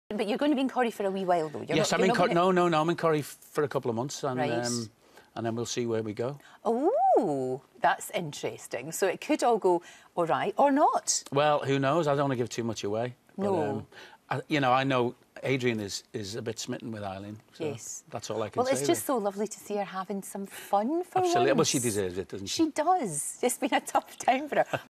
Mark Moraghan On How Long He'll Be In Corrie | Lorraine
Mark has recently joined the Coronation Street team and plays Adrian Mortimer. He tells Lorraine about how long he might be in the show for.